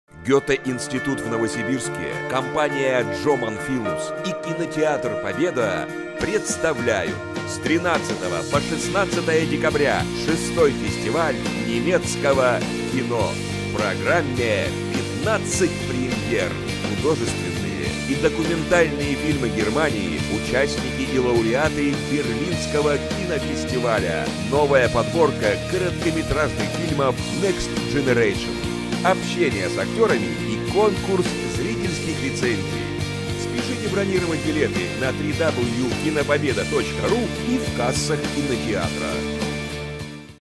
6. Deutsches Filmfestival
Von 13. bis 16. Dezember 2013 findet das 6. Deutsche Filmfestival in Nowosibirsk im Kino „Pobeda" statt. Zur Eröffnung wird „Finsterworld", das Spielfilmdebut der jungen Regisseurin Frauke Finsterwalder gezeigt.